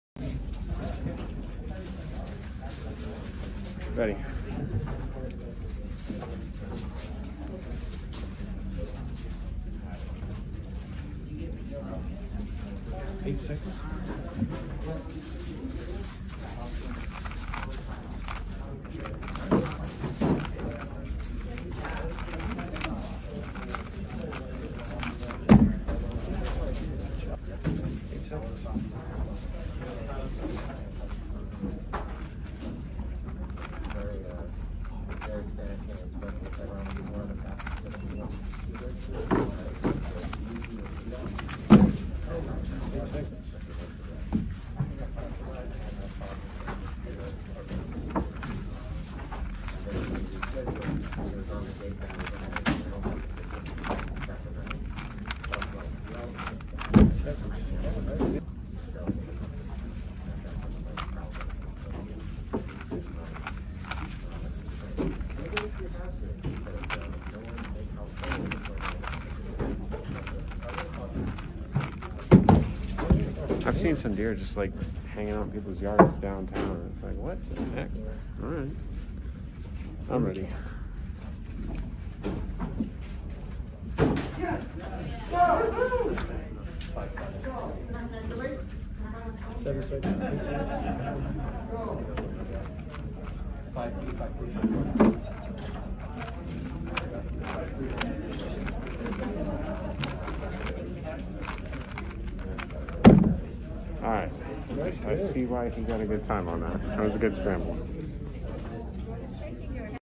11.14 Official 3x3 Average
I'm only uploading this to get the videos off my phone and I only kept these videos because Max has his WR avg reaction during my 5th solve.
Enjoy the bad camera quality from my iPhone :)
Done at Missoula 2021